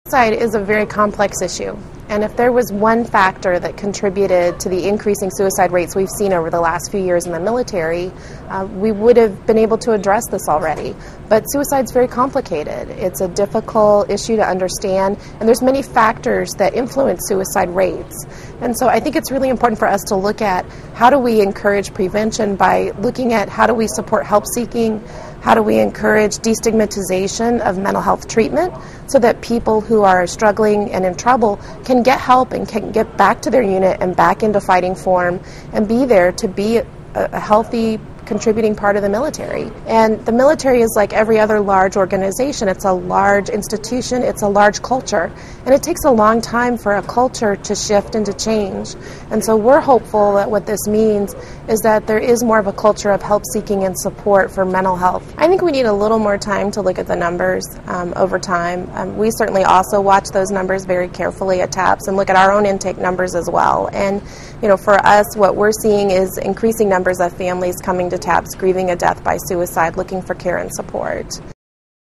Pentagon: Military Suicides Drop; Unclear Why
Defense officials say military suicides have dropped by more than 22 percent this year, but they're reluctant to give credit to detection and prevention efforts. (Nov. 12)